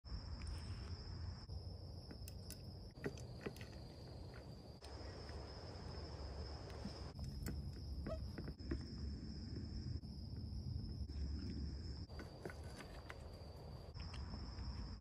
Raccoon Eats Spaghetti and Meat Balls short
A raccoon eats spaghetti with meat balls. Cute video